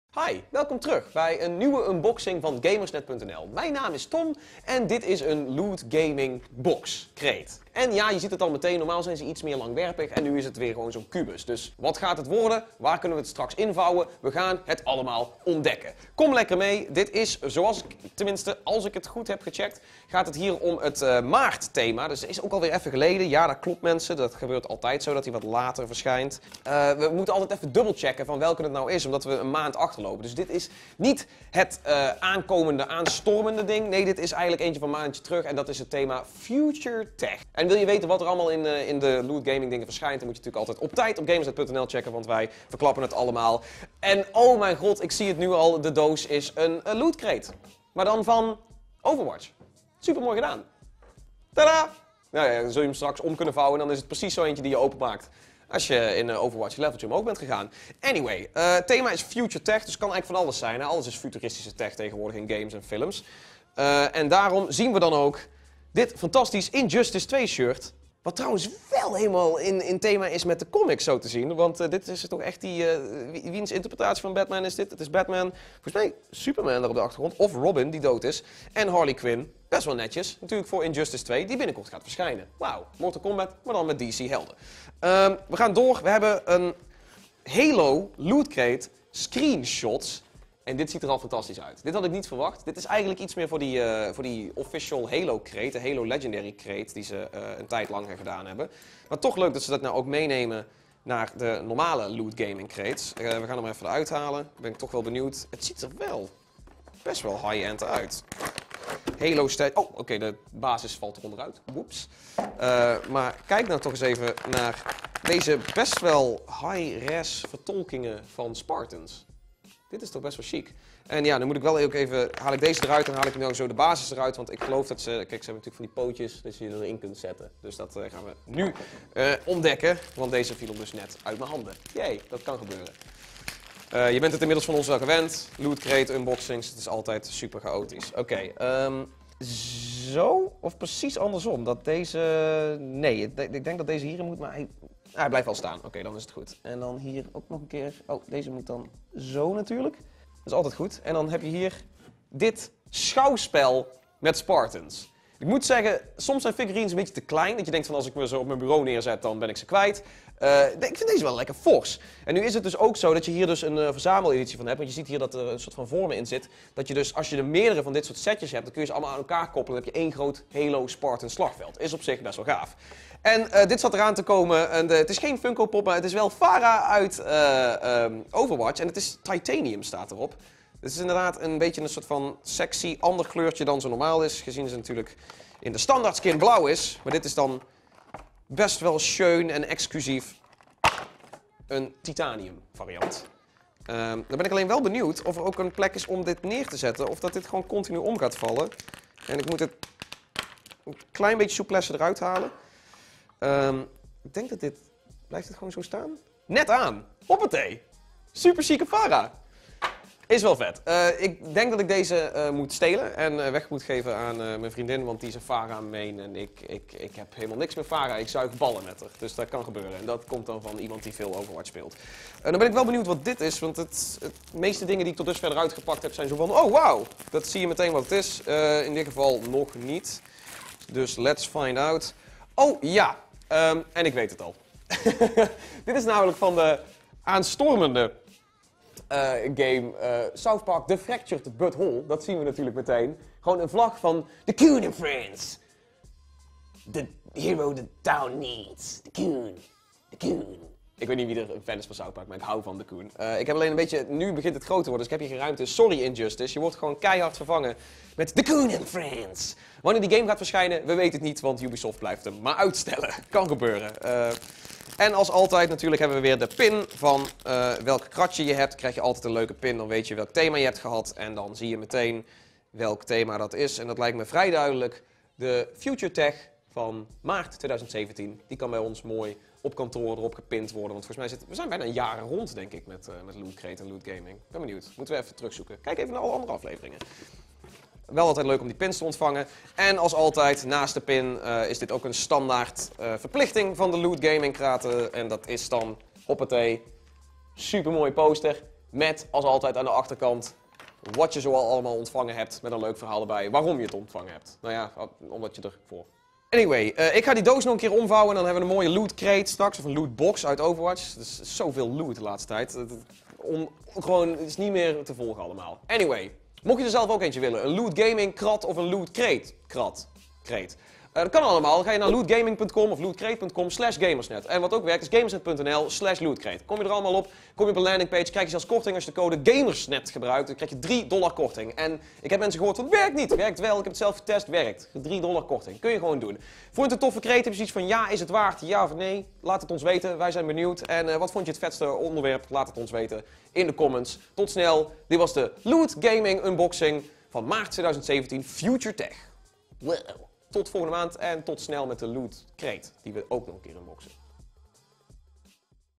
Loot Gaming April: Suit Up | UNBOXING
Zelf een Loot Gaming (of Loot Crate) doos scoren? Ga naar en gebruik de code 'GamersNET' voor drie dollar korting op al je aankopen!

Deze maand opent Tom het aprilthema van Loot Gaming, getiteld 'Suit Up'! Wat voor game-gerelateerde goedheid daar allemaal inzit, zie je in deze uitgebreide unboxing van GamersNET!